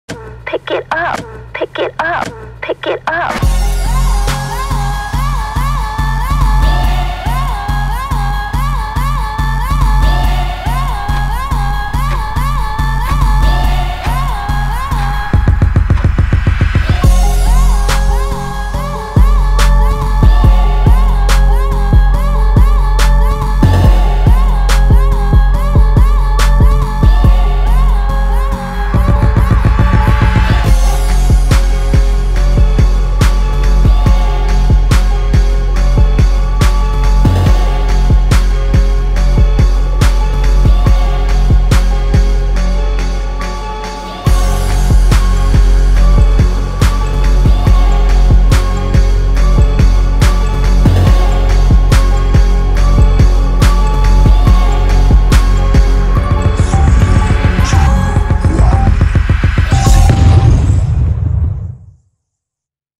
COUNTDOWN TIMER ( v 695 ) 1 min with wave music visualizer 4K
60 seconds countdown timer - audio visualizer